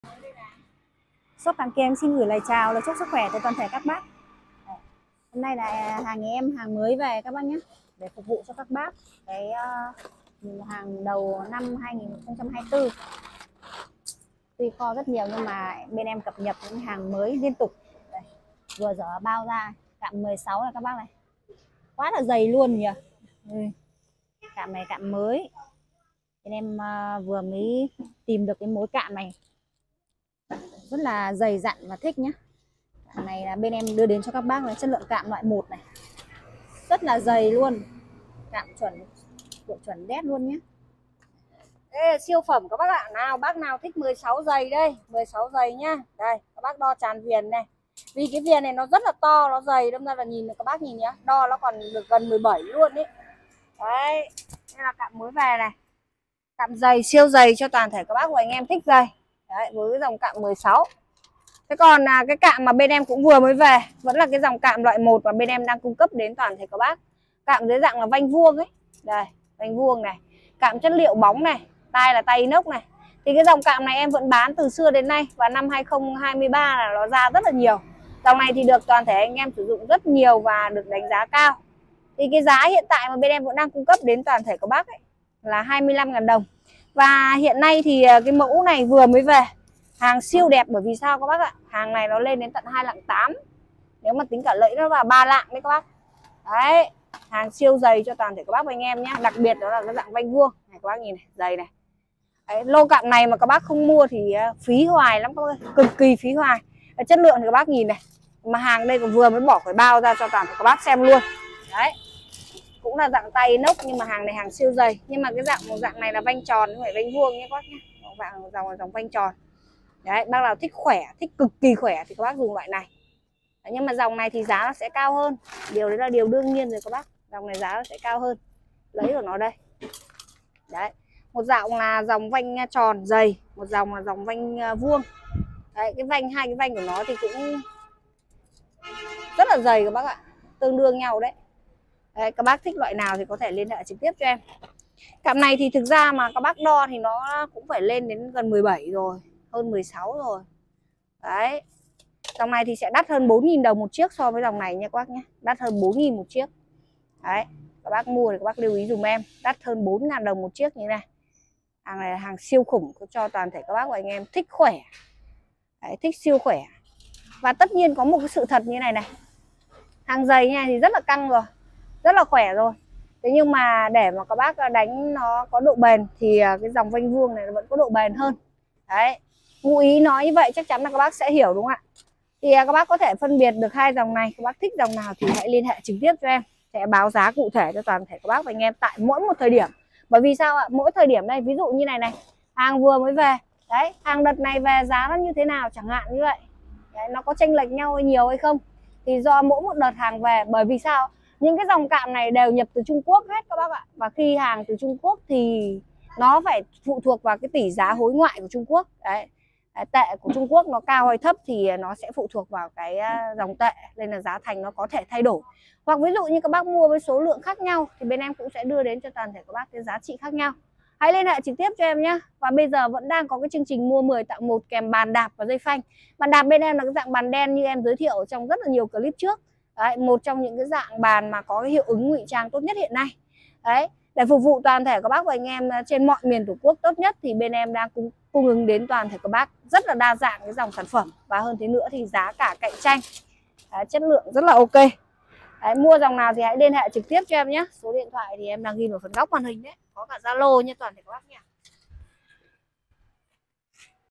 Bẫy cạm kiềng về thêm phục vụ cho năm mới bội thu.cạm 16 siêu dầy.chắc khỏe.bẫy từ 15kg a nha.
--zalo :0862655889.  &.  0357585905
+ Địa chỉ trực tiếp mua hàng: Bắc Giang